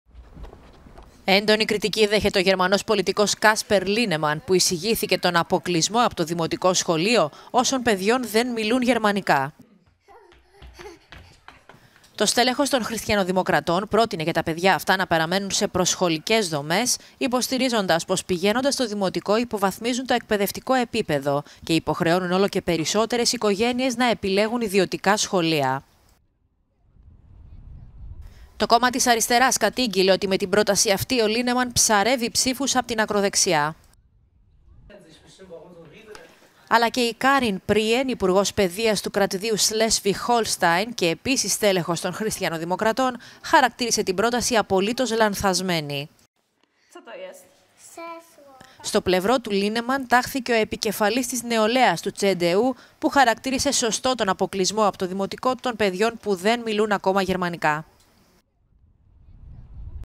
«Νάιν» σχολείο χωρίς γερμανικά
Αντιδράσεις στην πρόταση Λίνεμαν…